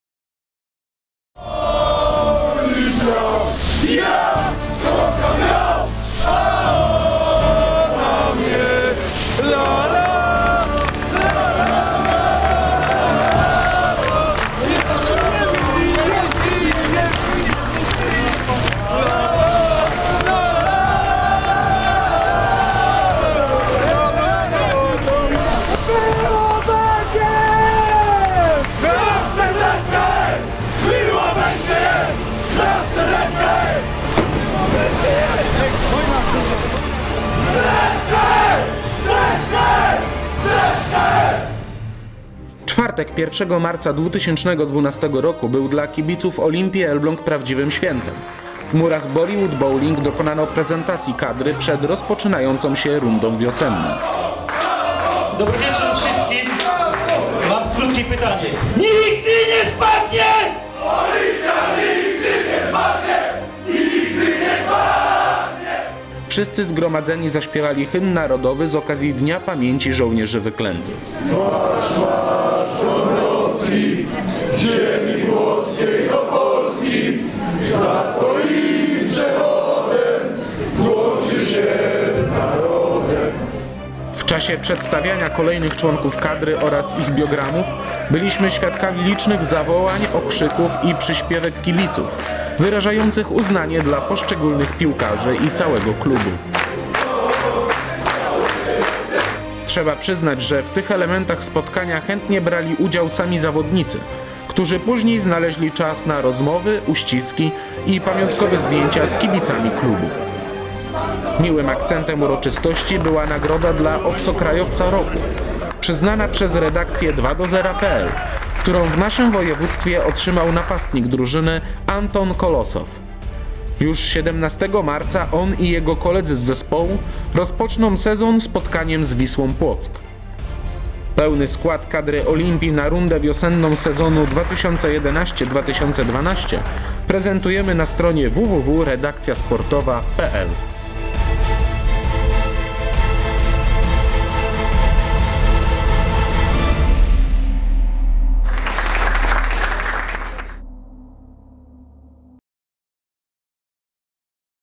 Olimpia Elbląg Prezentacja 2012
Zapraszamy do obejrzenia relacji filmowej z prezentacji zespołu Olimpia Elbląg, która odbyła się 1 marca 2012 w Bollywood Bowling.